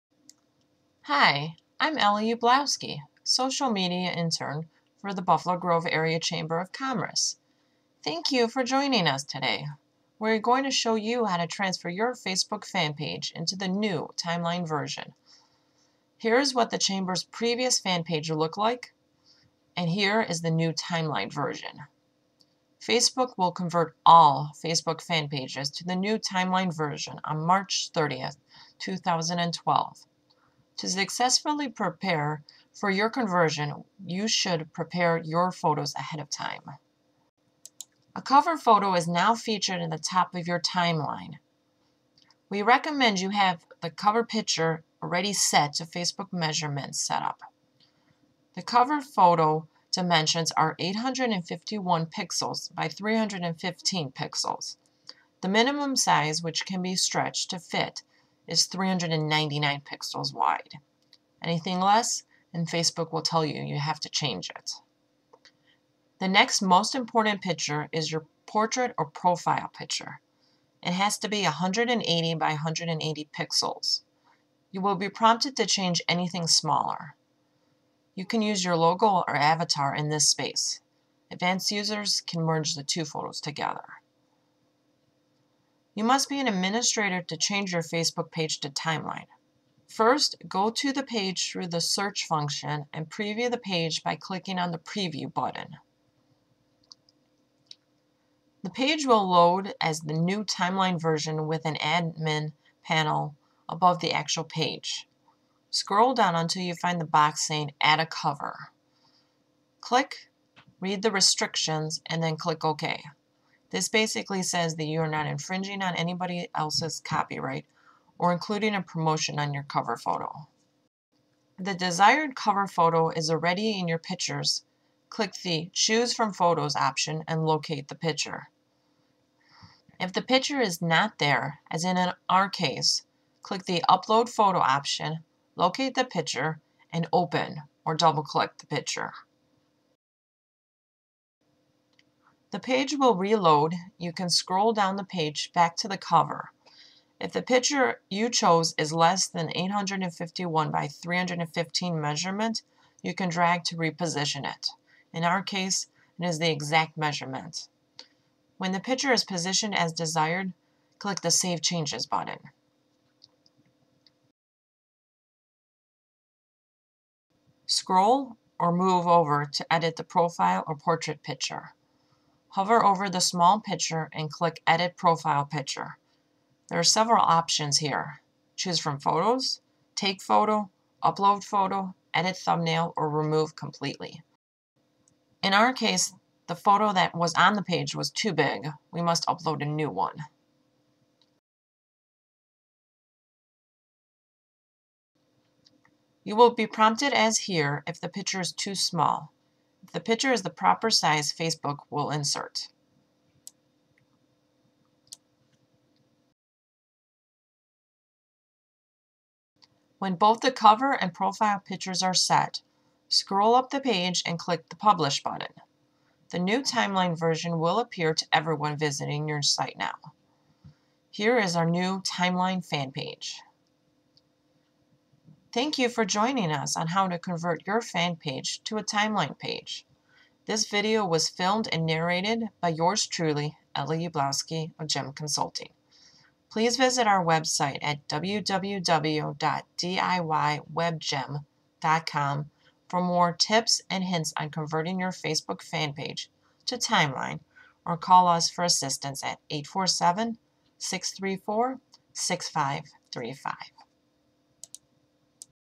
Convert Your Facebook Fan Page To Timeline - Main Photos by JEM Web Consulting
Ely shows you the first steps to convert a Facebook Fan Page into the new Timeline version.  This is an automatic conversion on March 30, 2012 unless you do it prior.  In this video she demonstrates how to change to the new version and how important photos are in presenting your company image or brand.  You should prepare your photos ahead of time to successfully prepare for this changeover.